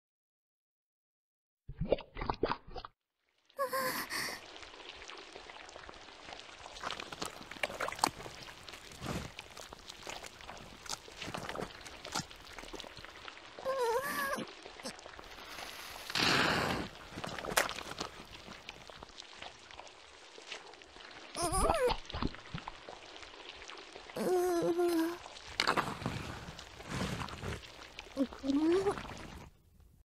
(Blender) Ruu Sink
Character belongs to QSSuwako, just wanted to make a short vid with her for fun.